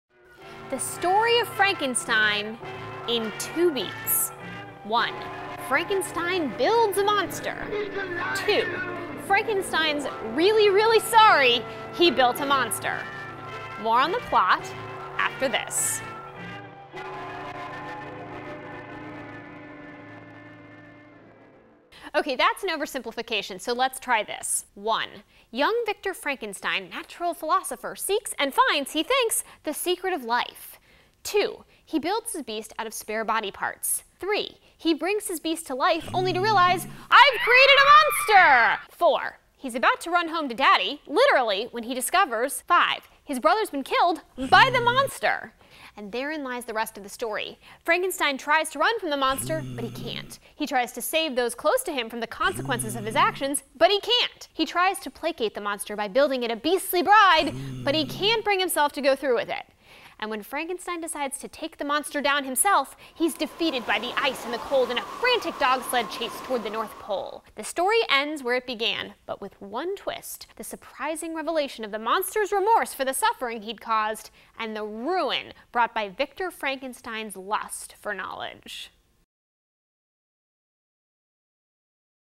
"Frankenstein" | Plot: Summary Analysis | 60second Recap®
"Frankenstein" by Mary Shelley. Plot Summary and Analysis. 60second Recap® Study Guide Video by Jenny Sawyer
In "Frankenstein," Mary Shelley crafted a plot that captivated millions, and served as the grist for dozens of Hollywood adaptations. But there's more to Shelley's story than a mad scientist and his killing machine. Indeed, the plot of "Frankenstein" haunts in more ways than one.